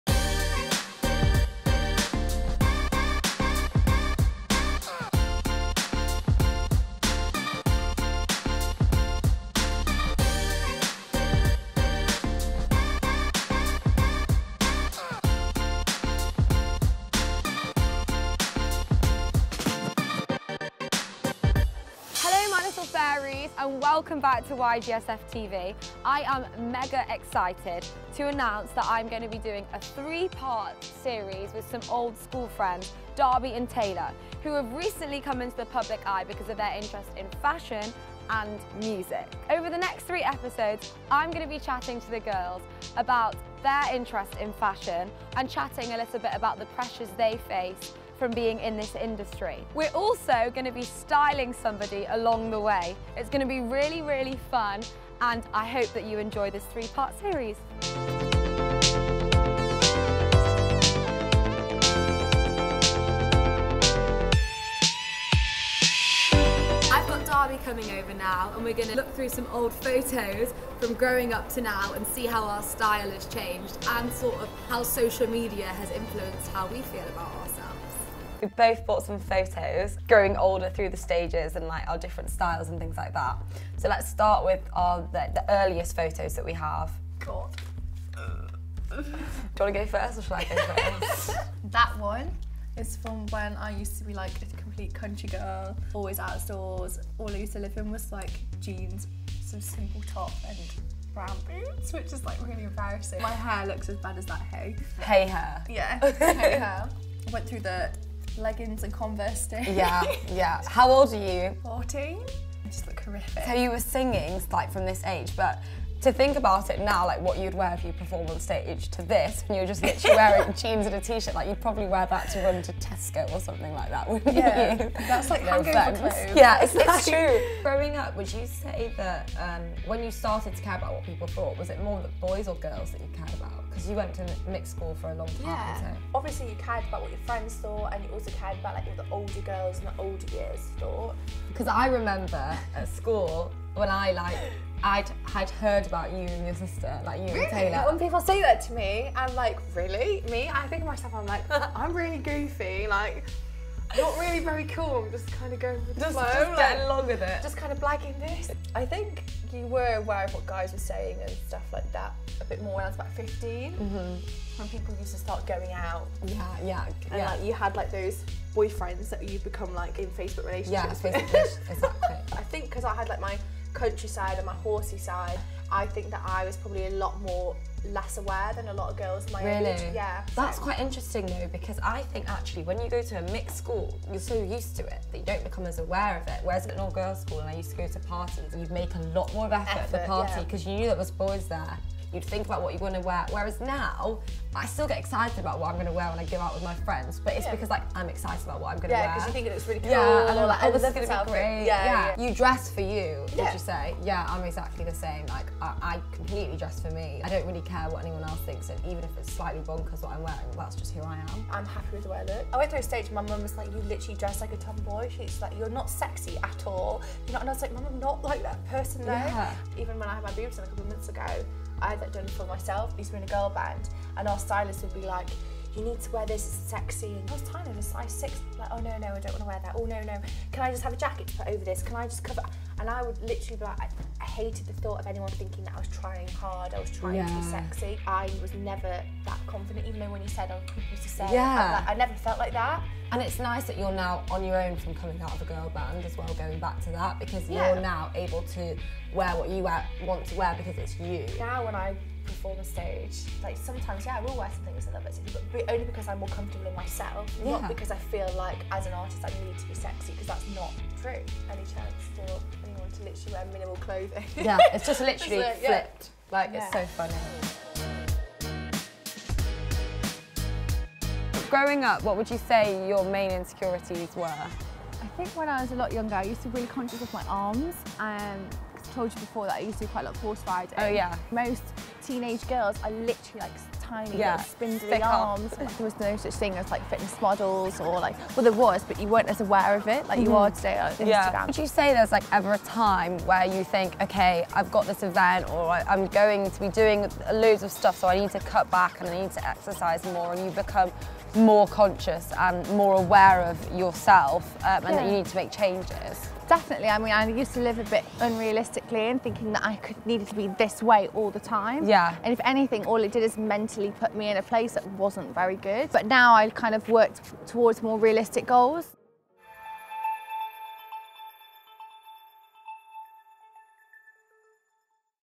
YG TV // Introducing Darby and Taylor Ward, The Sisters Exploding Onto The Fashion & Music Scene
In a 3 part series special I welcome Darby & Taylor Ward to the YG creative gang! I chat to the girls about their recent explosion into the music and fashion scene, as well as hearing about the pressures they face being on reality TV and constantly in the public eye. Episode 1 introduces Darby as she opens up about how insecurities impact her style...

A YG TV Production

Presenter: TALINA
Director: Douglas Rath
Production Company: MYTH FACTORY